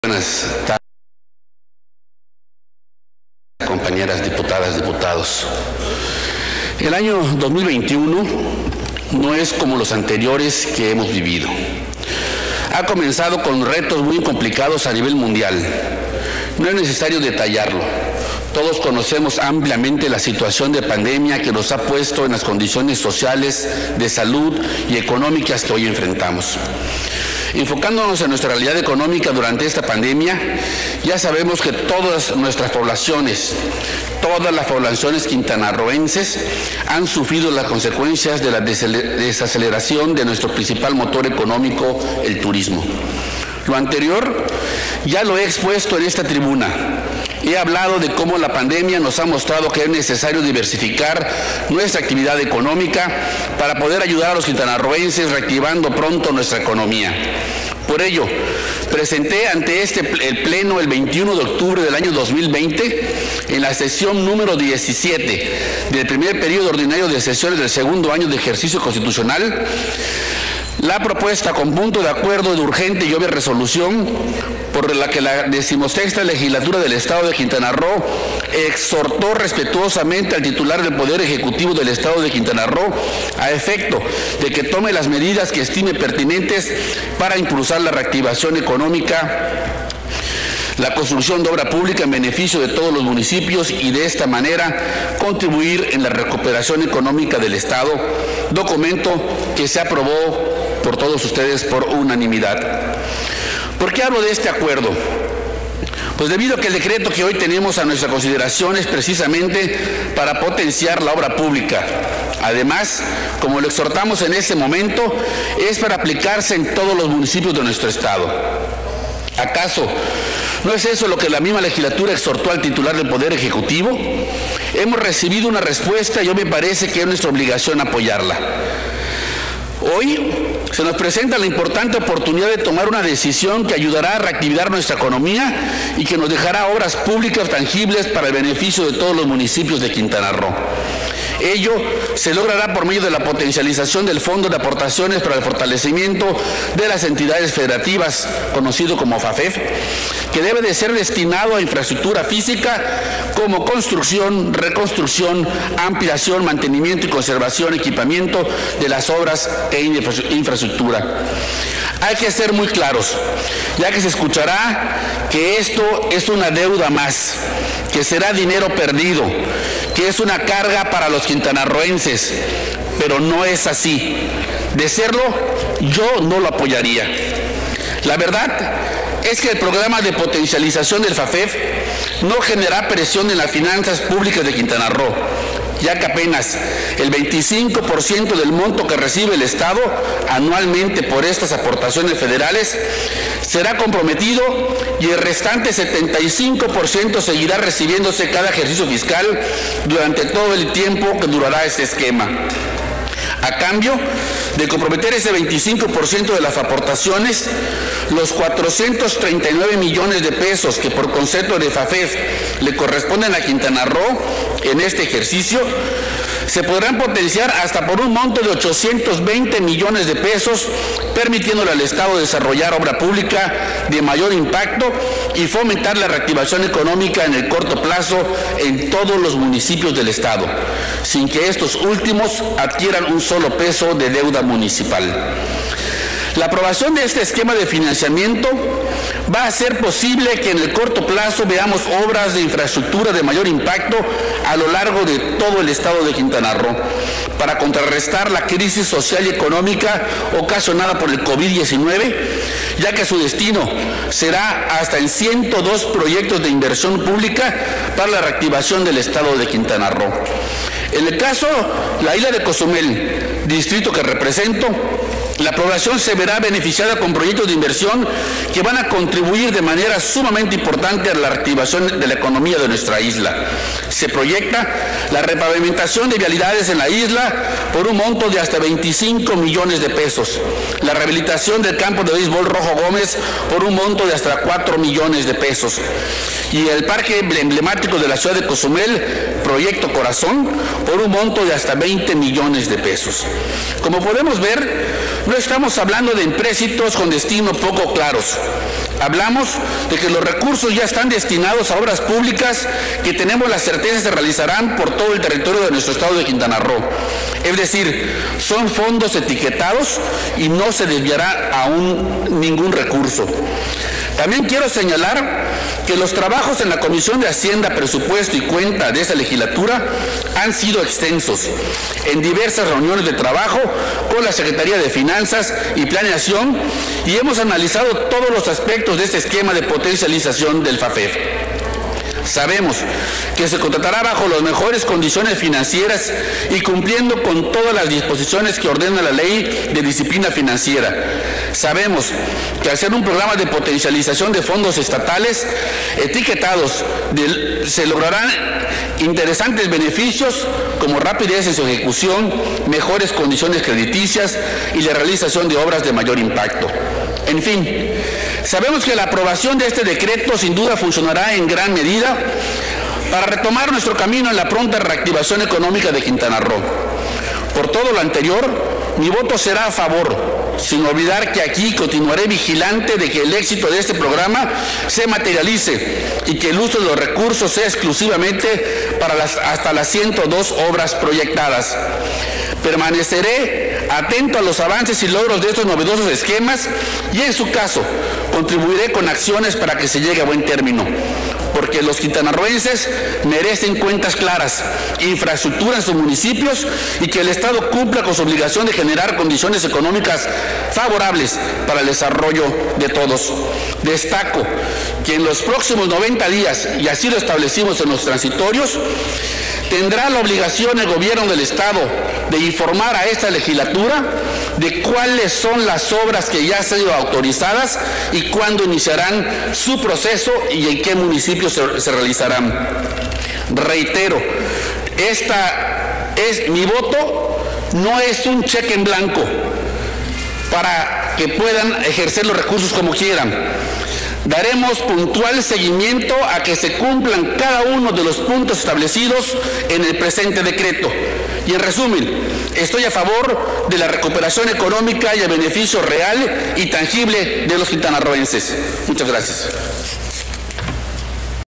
Intervención del Dip. Carlos Rafael Hernández Blanco. PRI (10/Mar/2021)
Intervención del Diputado Carlos Rafael Hernández Blanco, del PRI, en relación al Dictamen con Minuta de Decreto por el que la Honorable XVI Legislatura del Estado Libre y Soberano de Quintana Roo, autoriza al Poder Ejecutivo del Estado Libre y Soberano de Quintana Roo, por conducto de la Titular de la Secretaría de Finanzas y Planeación, a contratar por un plazo de hasta veinte años uno o varios financiamientos, con una o más instituciones financieras mexicanas, o través de la colocación de instrumentos de deuda en el mercado de valores nacional, directamente o a través de cualquier vehículo constituido para tal efecto y/o que actúe como deudor por un monto de hasta $820,000,000.00 (OCHOCIENTOS VEINTE MILLONES DE PESOS 00/100 M.N.)
Fecha: 1o de marzo de 2021